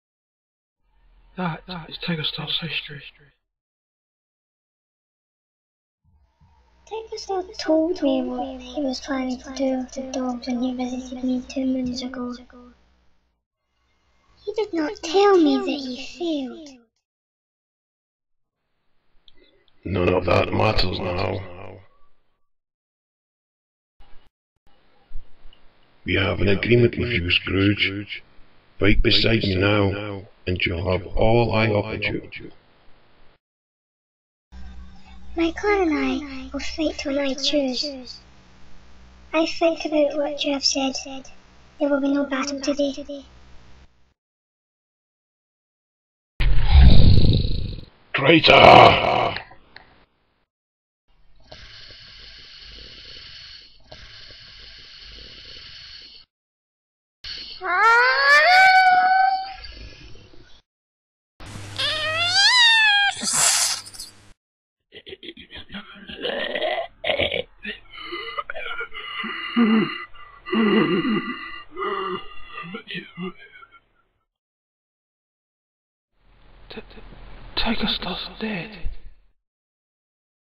TigerStar's Death (2009)
EDIT (20/2/16); 13 year old me was edge also end me

EDIT (23/5/11) : Right guys this video is old to me, but it's my most viewed video. Keep it up! :D

EDIT (15/11/09) : This is the WMM Edit. I can't get the Better Vegas Pro one up. The File is unreadable DX I'll find a way to update it!

I've been making this for the WeekEnd and I really like it X3

Yay for Watermarks!

Scene : TigerStar's Death
Book : The Darkest Hour

Voice Actors:
FireStar/Scourge : PikaPlatinum
TigerStar : My Dad

Sounds : PikaPlatinum

Programs Used :
MS Paint (Frames)
WMM/Sony Vegas Pro 9 (Editing and Music)
Audacity (Recording)

~Pika